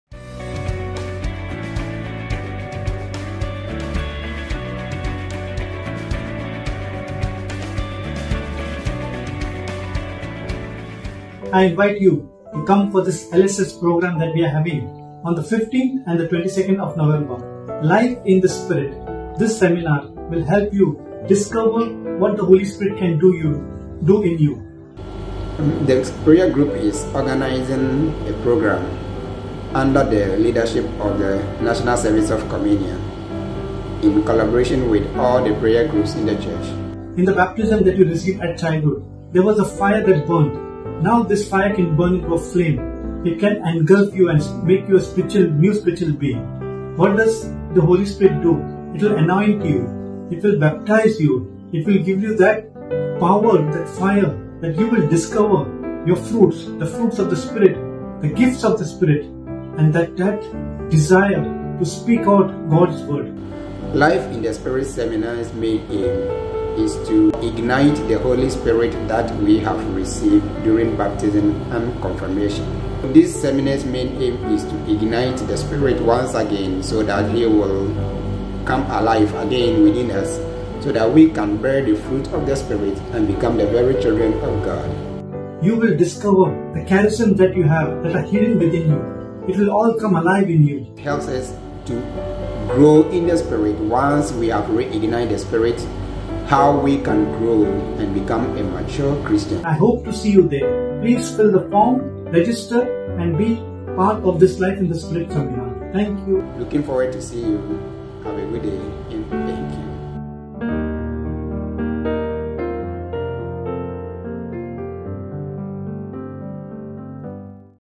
Life in the Spirit Seminar 2024_by Bro Augustine & Steven
Life in the Spirit Seminar 2024 to be held on Fridays, the 15th November 2024 and 22nd November 2024 at Our Lady of Rosary Church, Seraphic Hall, Doha-Qatar.